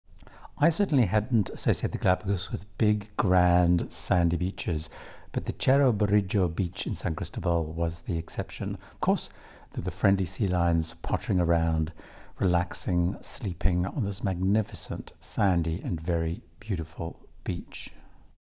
Hadn't Expected This! Galapagos Cruise Part 9
What! This beach in the Galapagos? Cerro Brujo on my Silversea Silver Origin Galapagos cruise